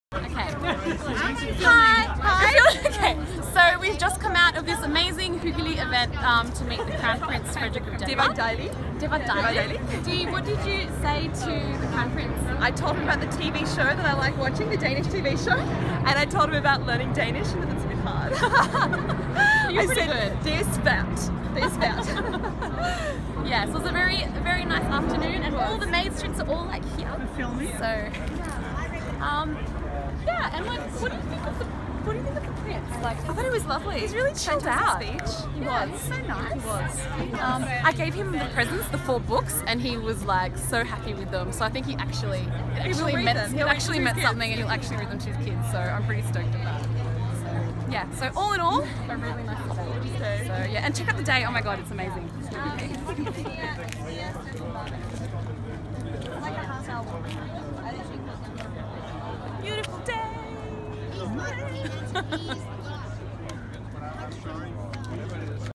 European Adventure Day 03 (bonus!) - Just met HRH Crown Prince Frederik of Denmark
With my Danish student Deirdre, giving our insight on meeting His Royal Highness Crown Prince Frederik of Denmark at Amalienborg Palace for afternoon tea!